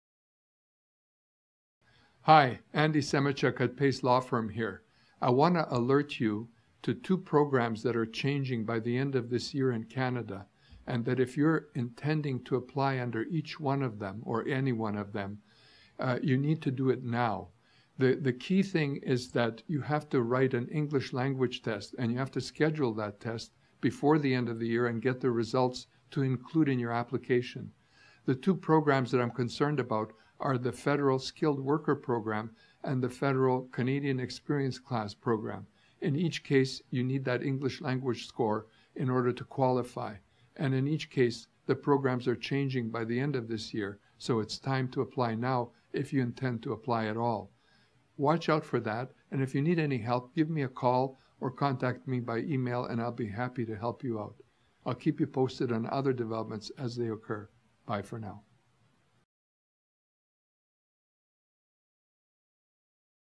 Canadian Immigration Rule Changes Coming in 2015
Changes to the Skilled Worker and Canadian Experience Class will be coming in 2015. If you wish to apply under current rules, get your English language (IELTS) test done right away and apply before the end of the year.